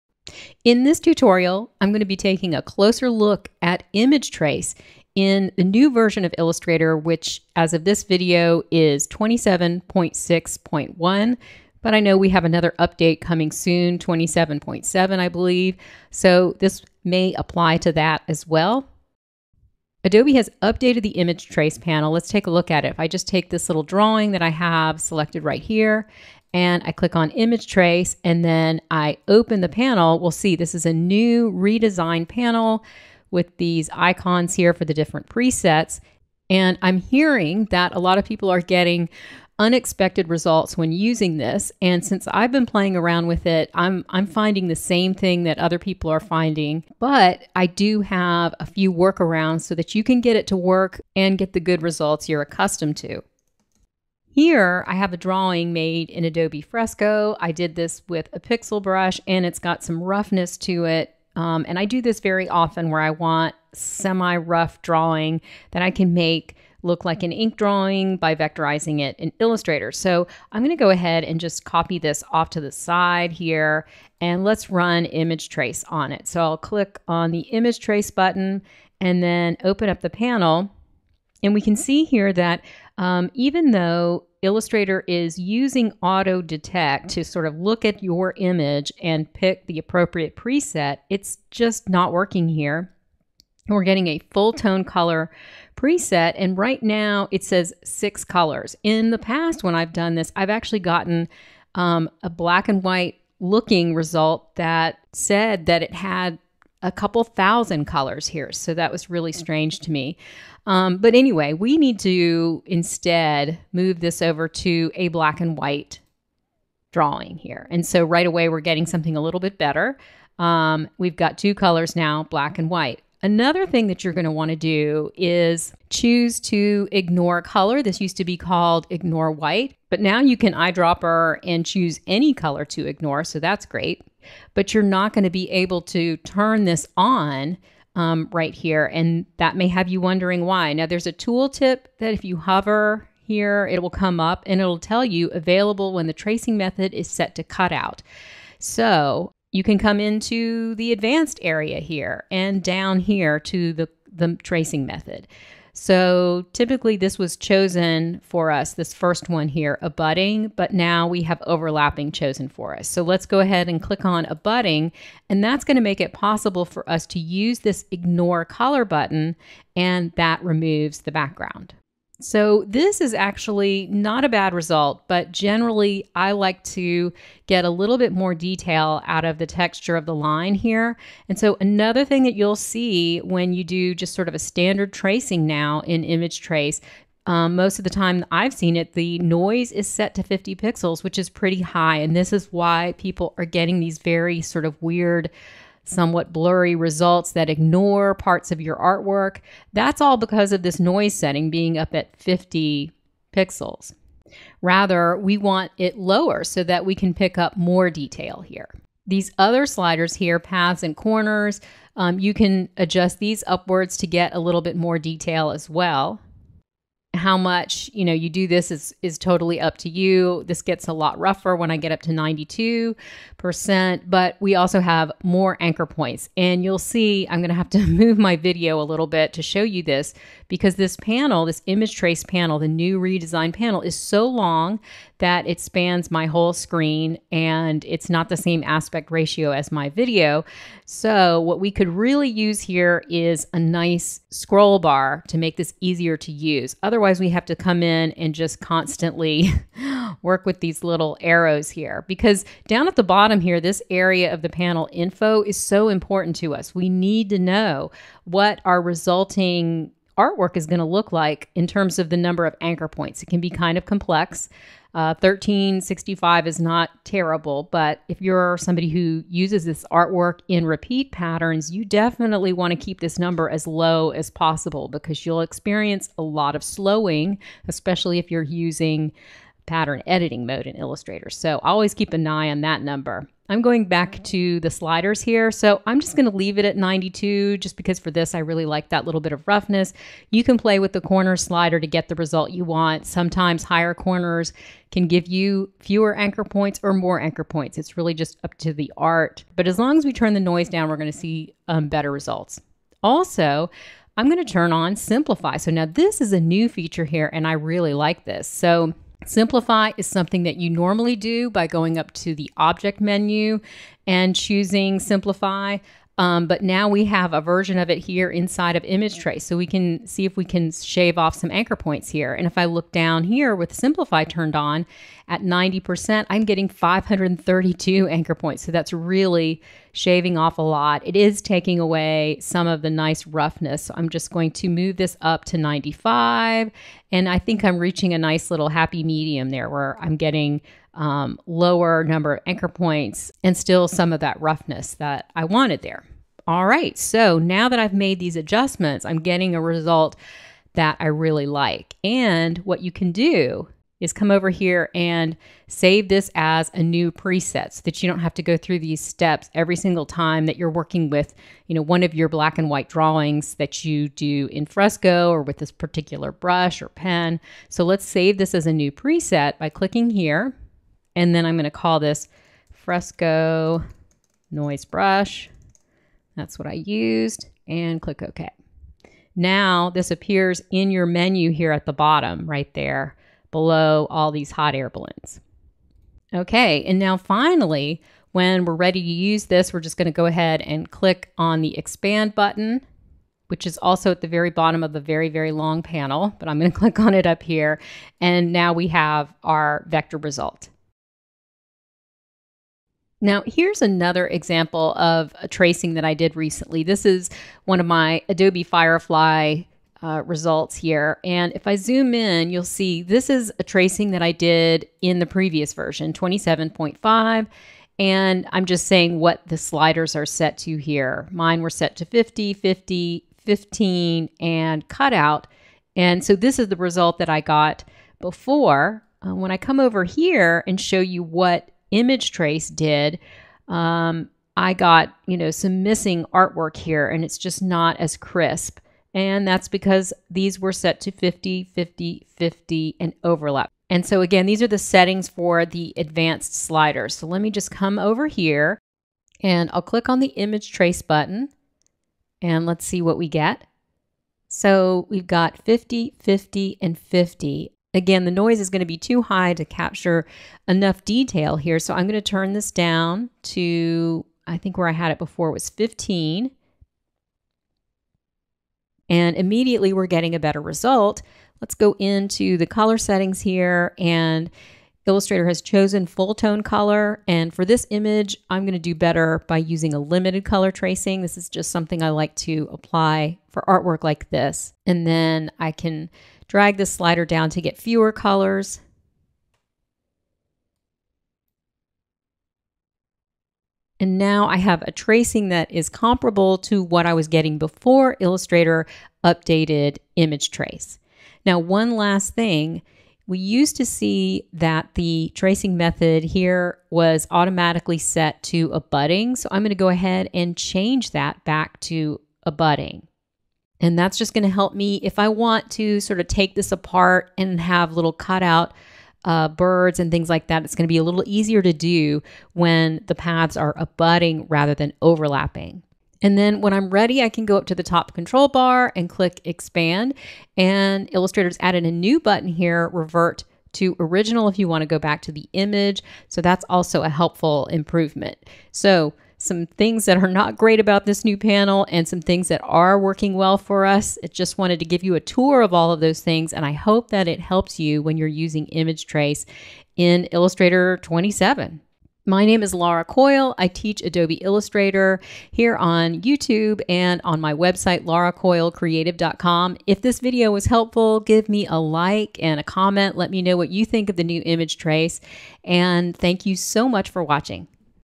What Happened to the NEW Image Trace in Illustrator 27.6.1??
Are you getting bad results from the NEW Image Trace panel in Illustrator 27.6.1? In this video, I share my tips for adjusting the settings to get better tracings. In the new version of Image Trace, auto-detect does not work as expected, and you can improve the results by working with the settings. Ignore White is now Ignore Color, which can only be enabled when your tracing method is set to Cut Out. The noise setting is set too high by default, and tracings look unfocused with missing details. No worries, with a few adjustments, you'll get more accurate tracings!

✏️LINKS